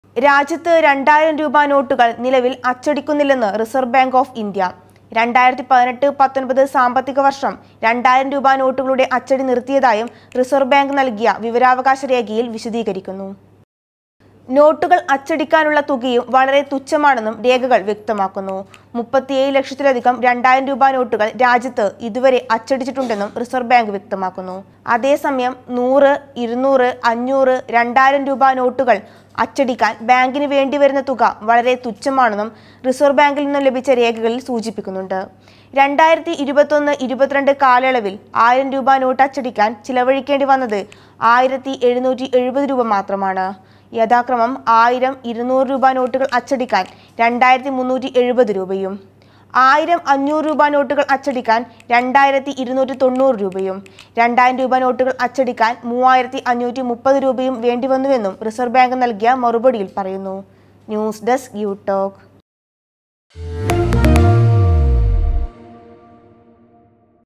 നിലവില്‍ രാജ്യത്ത് 2000 രൂപ നോട്ടുകള്‍ അച്ചടിക്കുന്നില്ല|youtalk
രാജ്യത്ത് 2000 രൂപ നോട്ടുകള്‍ നിലവില്‍ അച്ചടിക്കുന്നില്ലെന്ന് റിസര്‍വ് ബാങ്ക് ഓഫ് ഇന്ത്യ. 2018  19 സാമ്പത്തികവര്‍ഷം 2000 രൂപ നോട്ടുകളുടെ അച്ചടി നിര്‍ത്തിയതായും റിസര്‍ബാങ്ക് ബാങ്ക് നല്‍കിയ വിവരാവകാശ രേഖകയില്‍ വിശദീകരിക്കുന്നു.